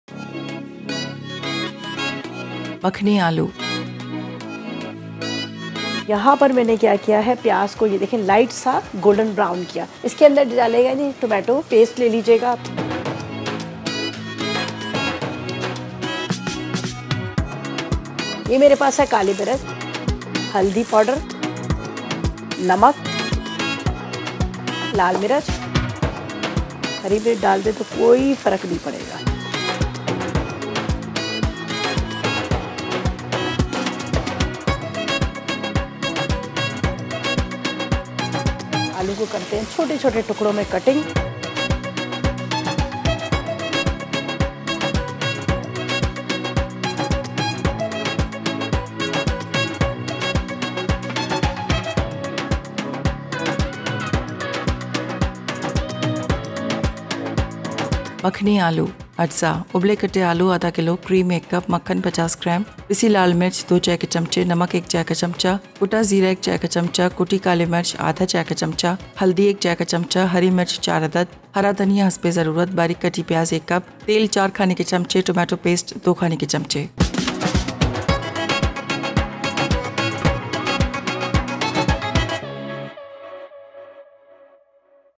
Makhni Aloo Recipe | Dhaba style Aloo Makhani Masala Curry Recipe | Chef Rida Aftab | MasalaTv
Makhni Aloo Recipe | Dhaba style Aloo Makhani Masala Curry Recipe | Chef Rida Aftab | MasalaTv @MasalaTVRecipes

Makhni Aloo Written Recipe :

Ingredients:
Boiled sliced potato                               ½ kg
Re chili powder                                        2 tsp
Salt                                                             1 tsp
Crushed cumin                                         1 tsp
Thinly sliced onion                                    1 cup

Recipe:
First heat the oil and butter and fry the onion. Now add all the spices and tomato paste and cook it for a while. Then add the boiled chopped potatoes. Now add half a cup of water and cover it. Finally, add cream, green coriander and green chilies and  switch off the stove.